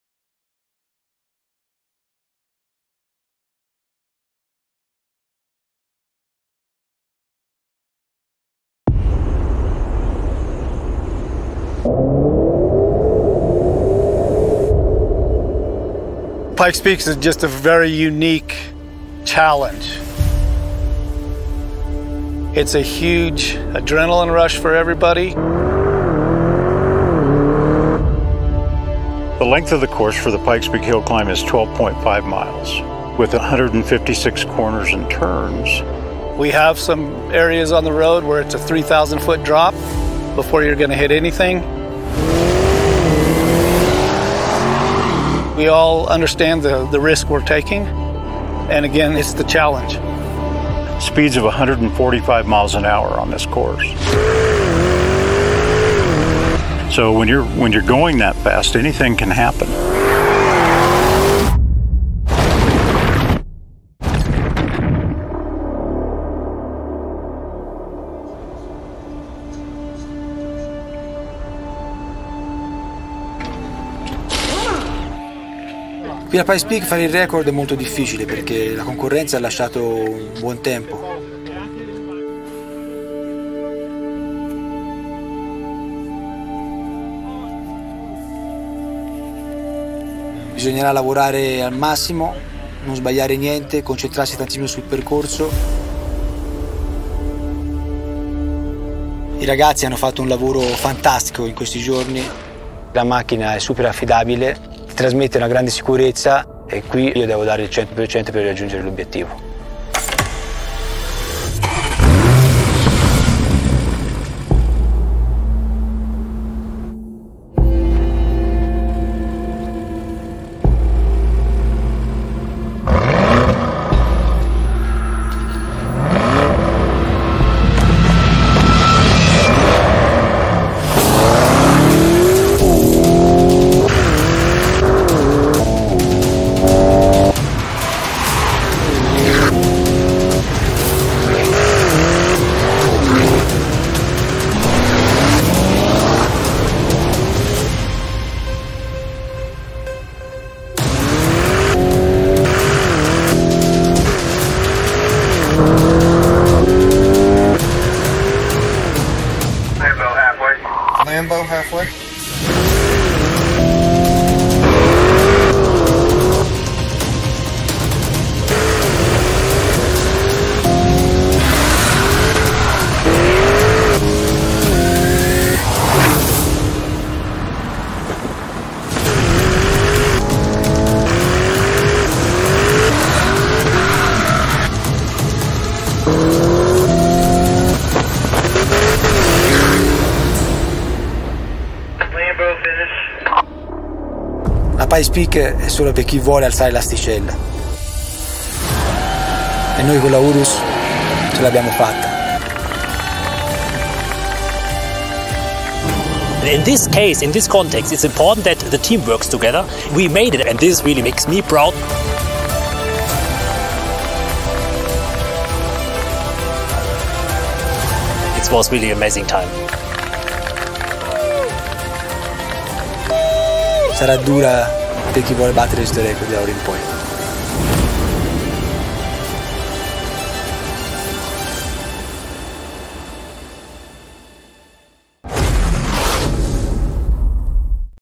Lamborghini Urus Pikes Peak record
The new Super SUV model covers 20 km and 156 turns in 10:32.064

Ahead of its premiere in mid-August, the yet-to-be-revealed new Urus model set a new record in the production SUV category on the track used for the Pikes Peak International Hill Climb: the famous uphill race that recently celebrated its 100th Running.

Hillclimb champion and Pirelli test driver Simone Faggioli, holder of the rear-wheel drive record set in the 2018 ‘Race to the Clouds’ on Pikes Peak, was entrusted with the new Urus, sporting a camouflage wrap ahead of its official presentation. The timed attack took place on the same route as the race, with the road closed to traffic for the occasion. Starting from an altitude of 2862 meters, Faggioli tackled the 156 turns that wind for 20 km (12.42 miles) ascending a further altitude of 1439 meters over an average gradient of 7%, and crossing the finish line at 4302 meters (14,115 ft) in 10:32.064.

Yet to make its formal debut, the new Urus was not entered in the official race but the time was recorded by the official timekeepers of the Pikes Peak International Hill Climb. The Urus bested both the previous PPIHC sanctioned non-race day manufacturer record of 12:35.610 set by Paul Dallenbach in 2014, and the current race day record of 10:49.902 held by Rhys Millen, set in 2018, in a Bentley Bentayga.